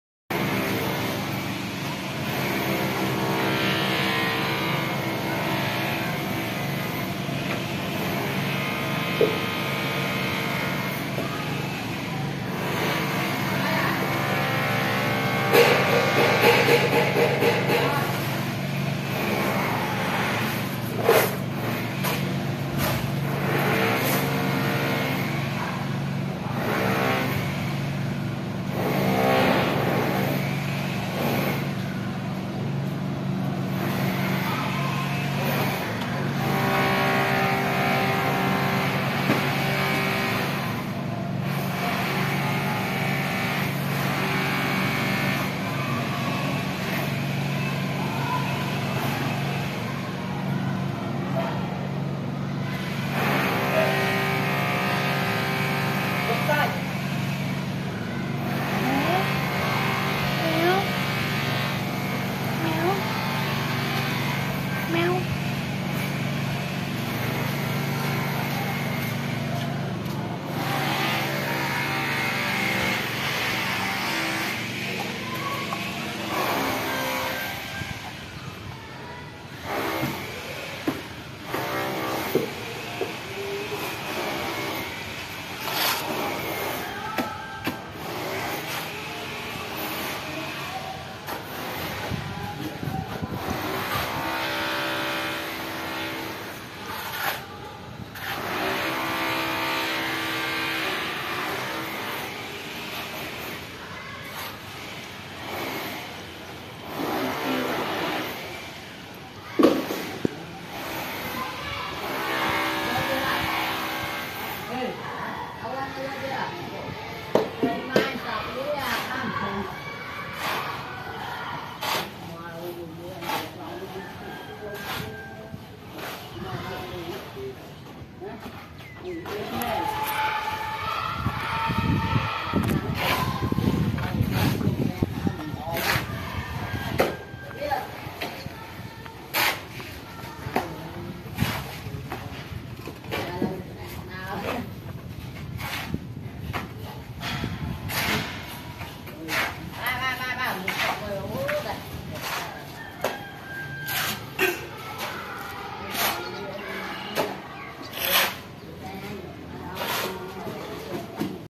Cats and Kittens Drinking water and Walking around the pagoda | Cat and kittens Pagoda
This channel we talk about lifestyle of all cats and kittens who live in pagoda. We try to feed cats and kittens.